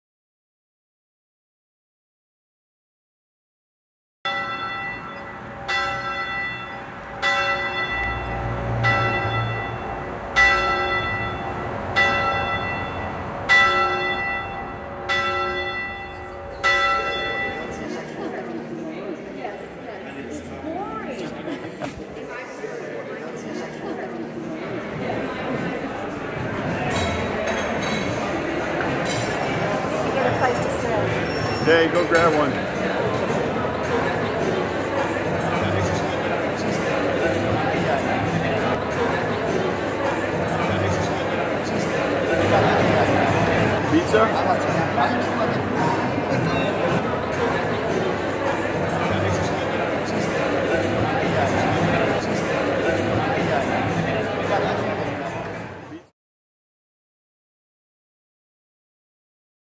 Holy Trinity Brompton
Welcome the bells calling the worship service to begin at Holy Trinity Brompton in Queen's Gate. The narrative of this video is found in the coffee table book, "The Curious Tales of Scott & Petey, Lost in England 2024."